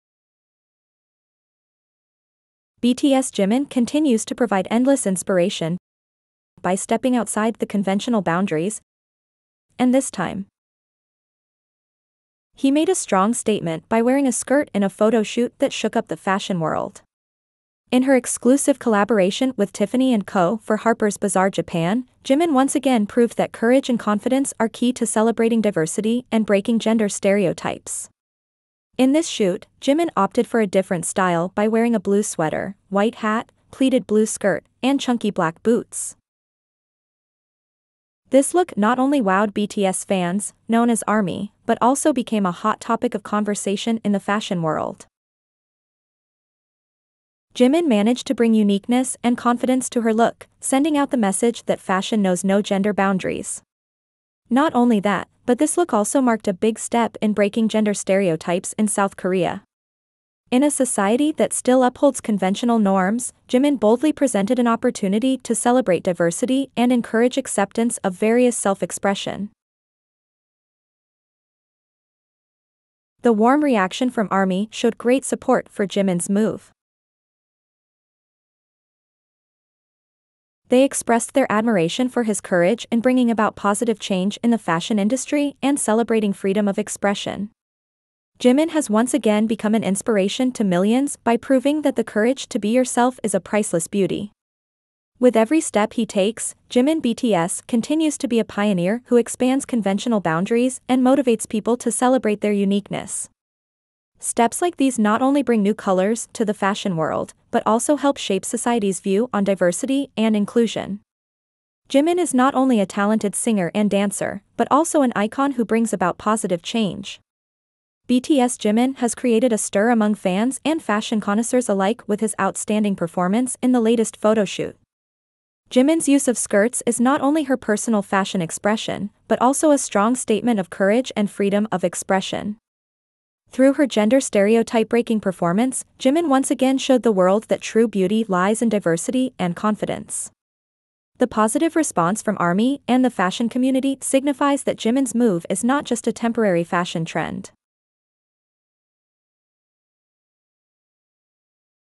Shut up and don't bother her with a skirt!!! This is the perfect look for BTS jimin ???
Get the ultimate fashion inspiration from BTS Jimin with this perfect look featuring a stylish skirt. Shut up and don't bother, this outfit is all about confidence and style. Watch the video now to see how you can recreate this look and rock it like Jimin!